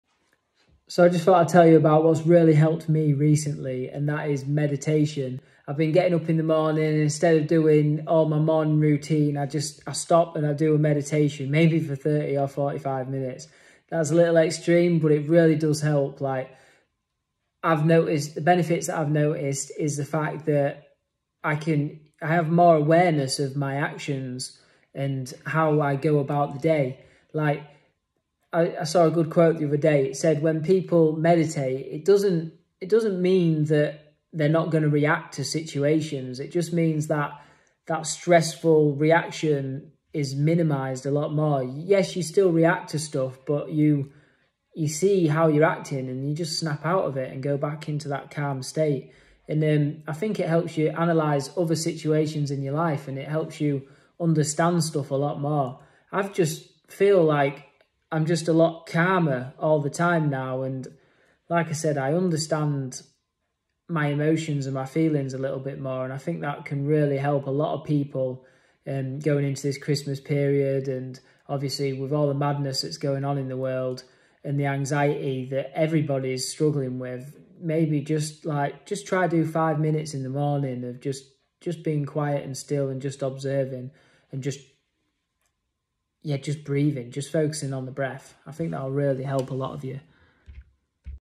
How meditation will help you
How meditation can help you...
I have so much more mental clarity and I want you to have too!
This video is to help persuade you to start meditating, in the morning and truly experience all the benefits and feel so much better this December.

Please just try focus on deep belly breathing for 30 seconds, then carry on if you.
Just 30 seconds of deep conscious breathing can bring you back into rest and digest and the calm parasympathetic state

Social media @jamescookcoulson

*Also known as Bowel Restore in the USA*